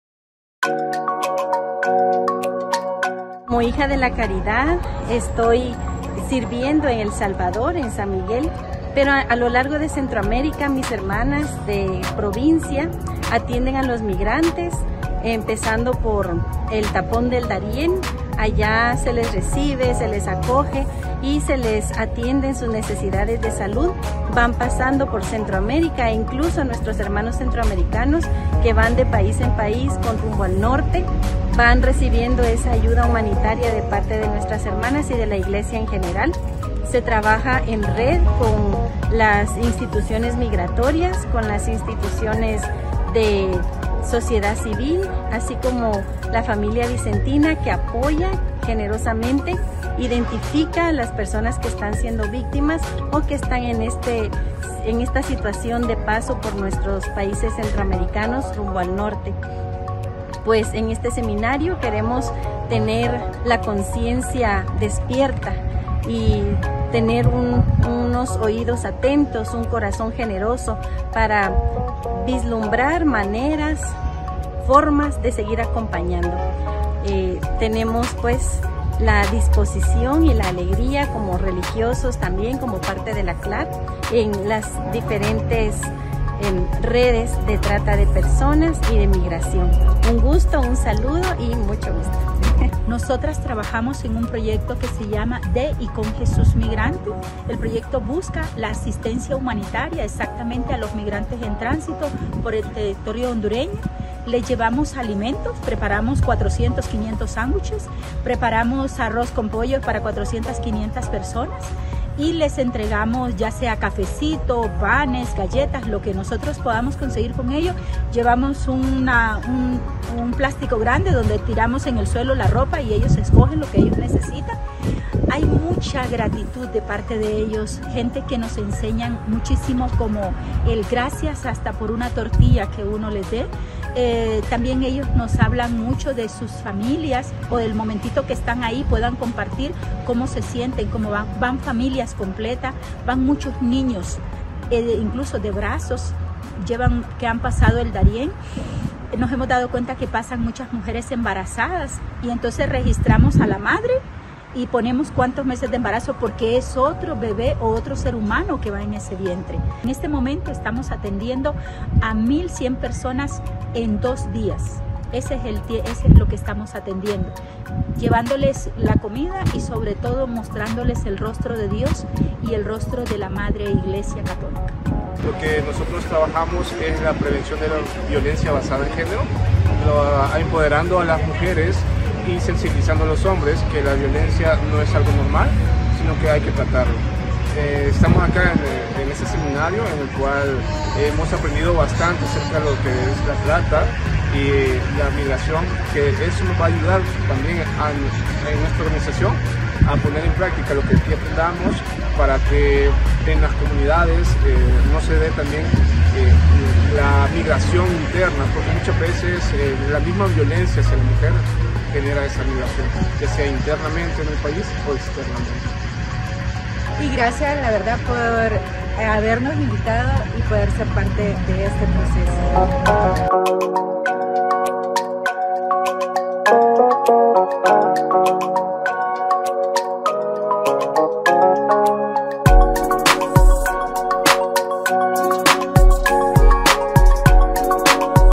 CLAR - Voces de la Vida Religiosa - Migración y contra la Trata de Personas
La VidaReligiosa que peregrina en AméricaLatina y el Caribe acompaña el caminar de las/os migrantes que recorren las fronteras del Continente, y une sus esfuerzos contra el Tráfico de Personas.